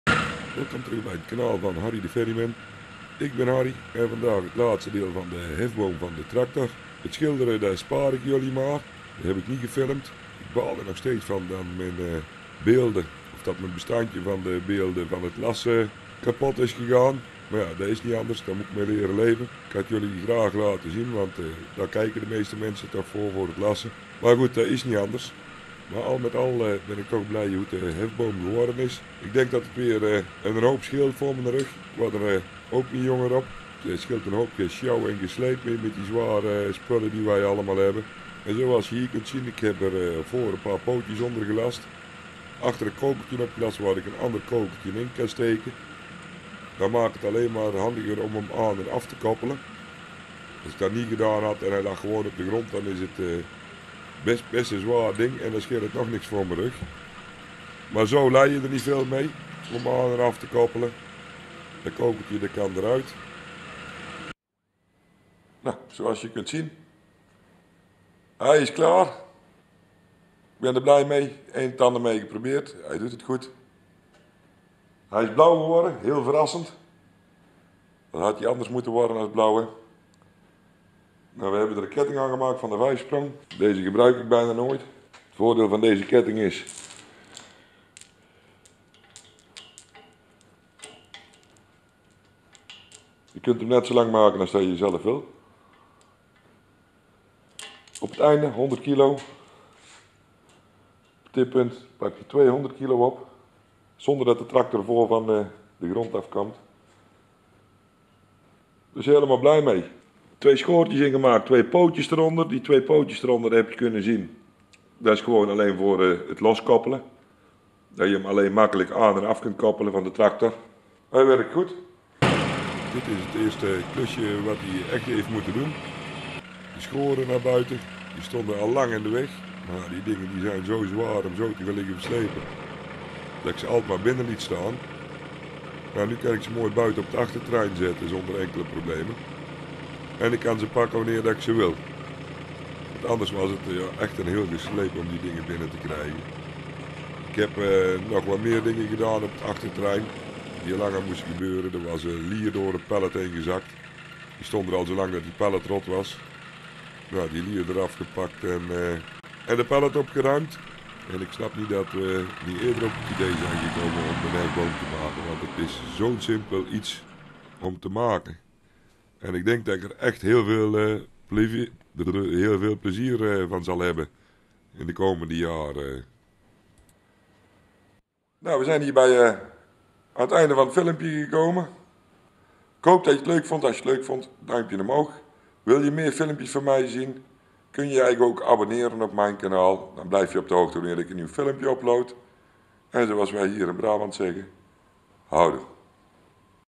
Een hefboom voor de tractor maken hoe doe je dat deel 3
in deze film maak ik een begin aan de hefboom voor de tractor. het principe berust op de Harry Ferguson crane tegenwoordig heb je wel hef bomen die in de hefinrichting kunnen maar die werken allemaal met de top stang eraan.met deze constructie is de beweging van de boom volgens mij veel groter.